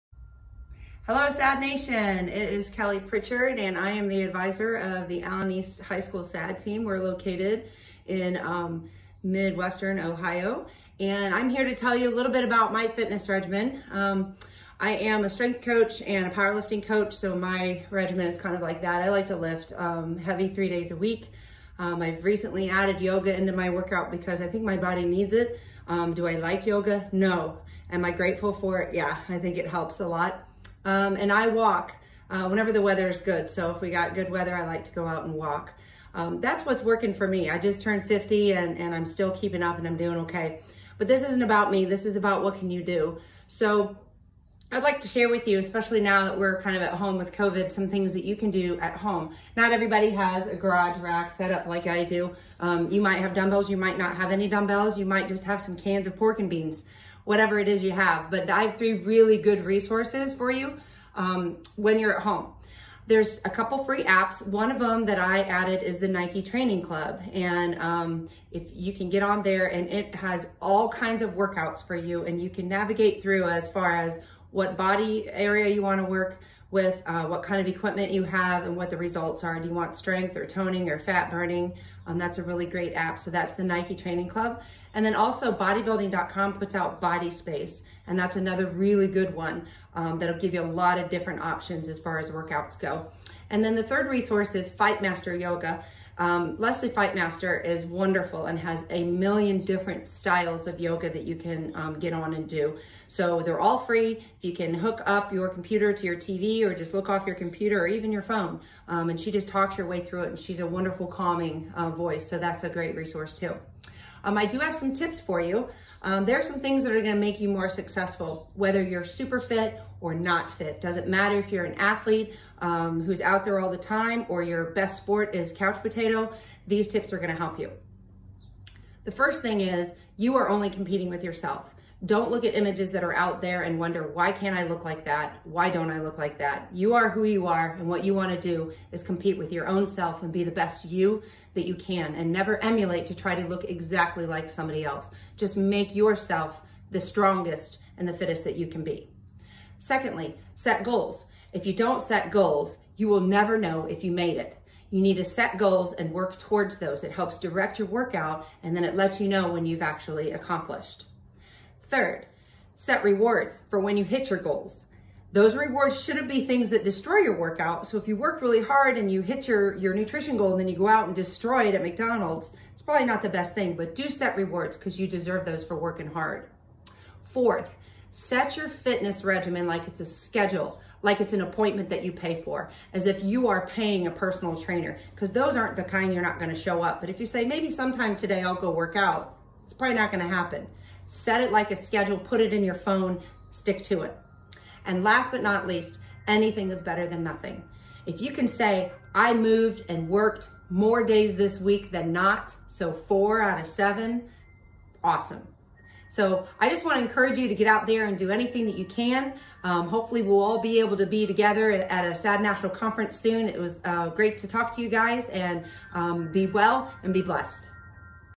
WellnessWednesday with Kelly Prichard!
Working out is about so much more than getting fit. It's about motivation & persistence to create a regimen, compete with yourself, & achieve your goals to be the strongest and the fittest that you can be. Get up & get moving, SADD Nation! 💪🏼